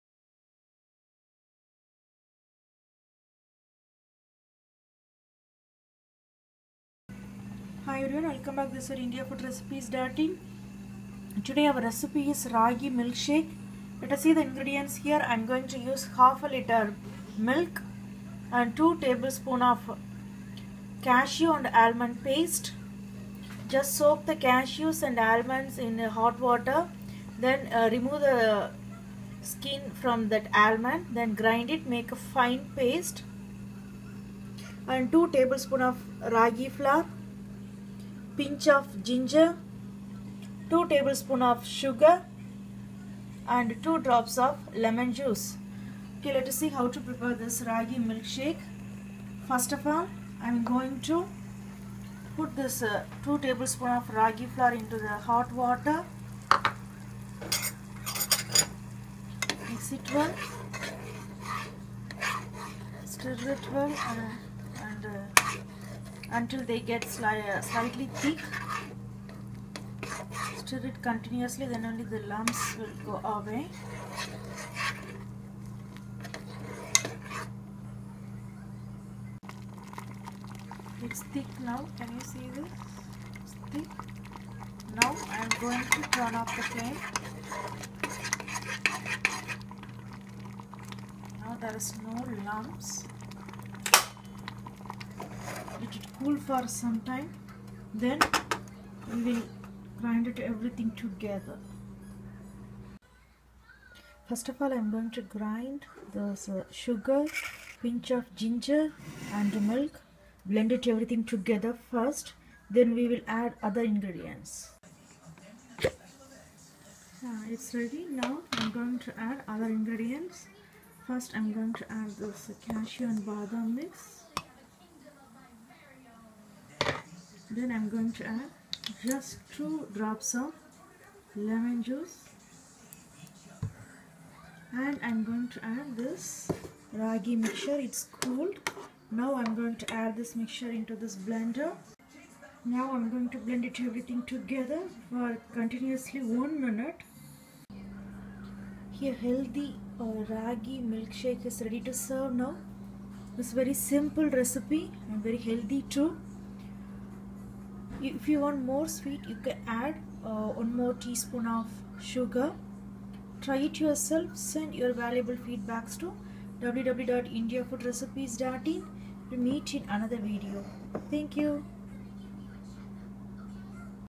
Ragi Milkshake - finger millet milk shake
shows you how to prepare healthy raagi milkshake recipe in simple way.